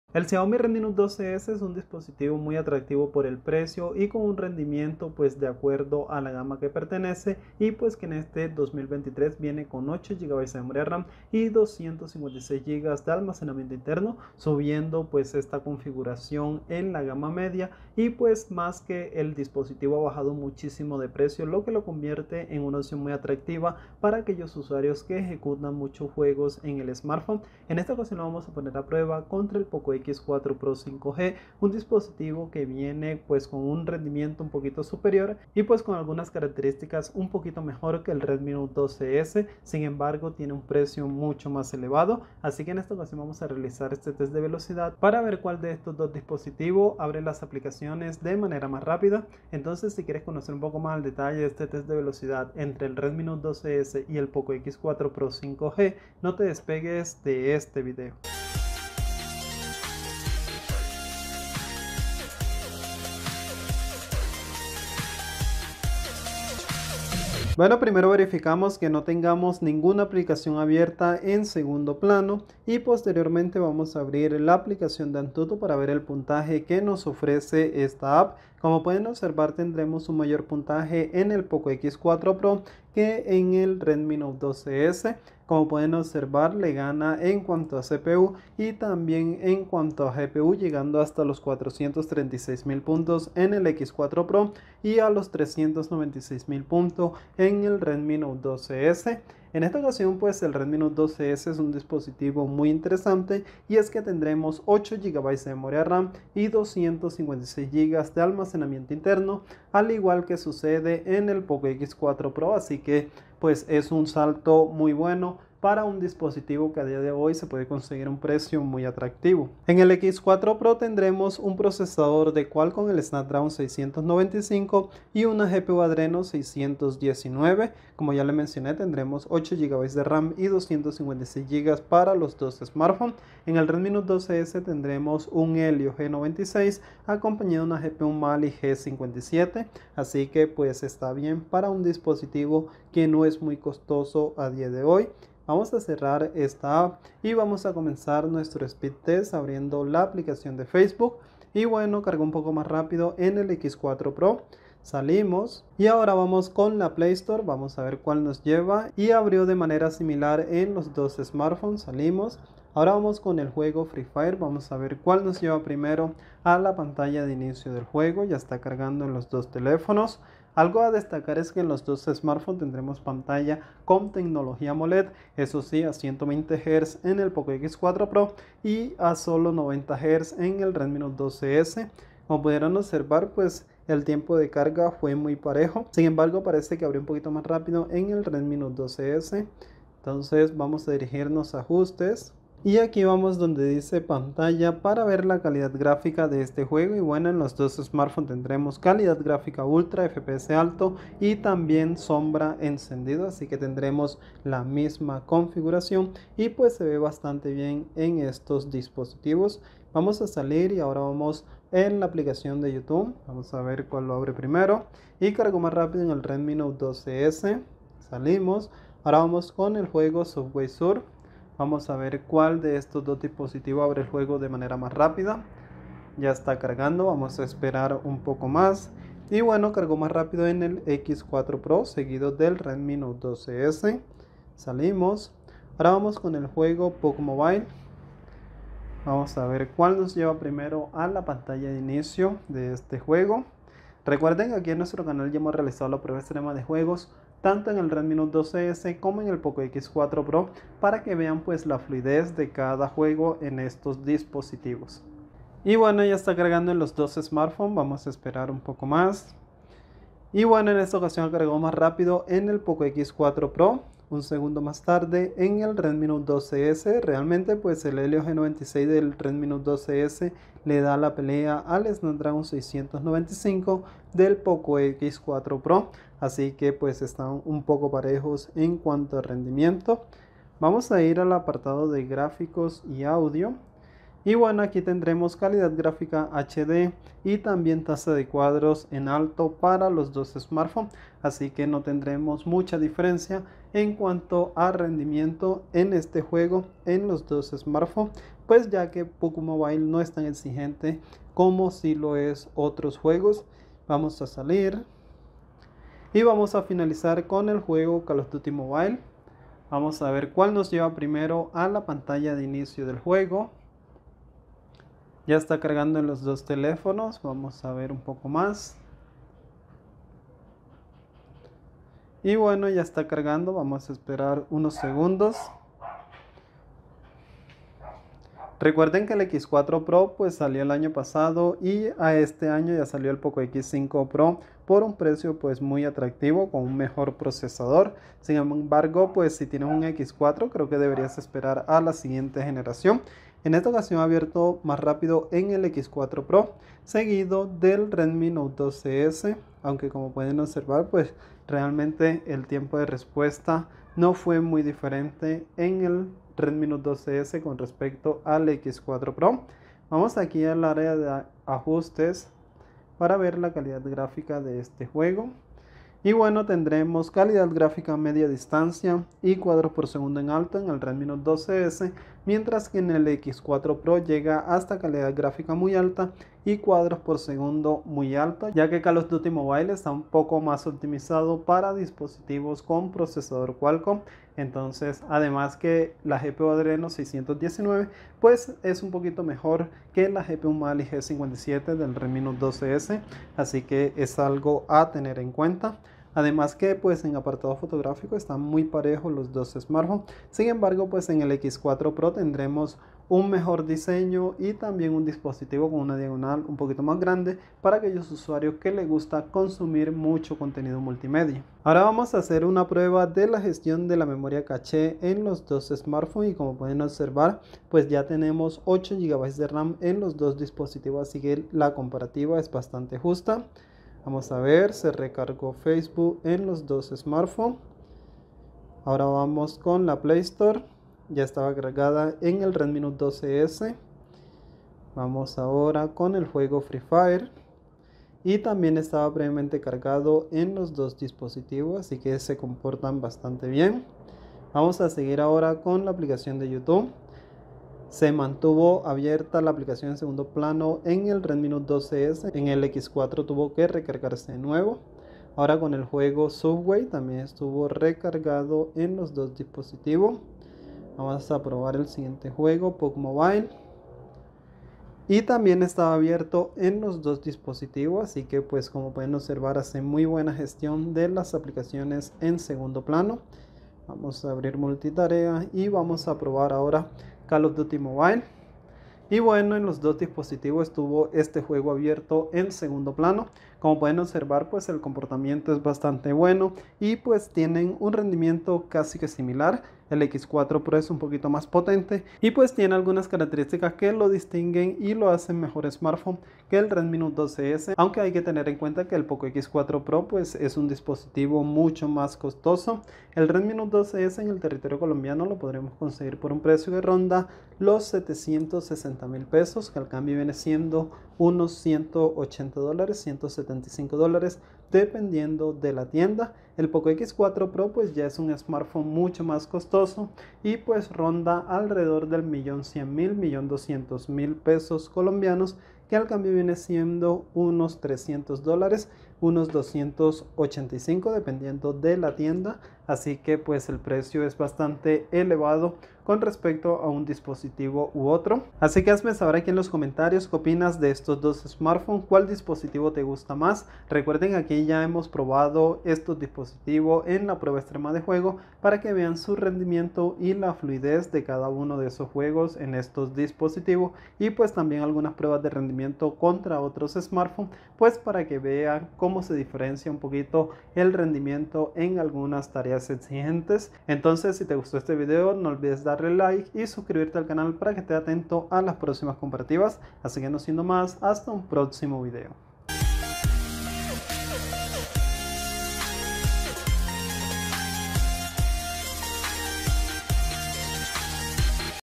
Speed Test REDMI NOTE 12S vs POCO X4 PRO 5G🔥 Helio G96 vs SNAPDRAGON 695 😱Cuál es más rápido?🔥
En este video realizamos un Test de velocidad entre el POCO X4 PRO 5G & El Redmi Note 12s, para que decidas cual es mejor opción comprar.

El Xiaomi Redmi Note 12S con Helio G96 en su versión de 8gb de RAM y 256gb de almacenamiento interno con un precio de 760.000 COP unos 185 dólares al cambio.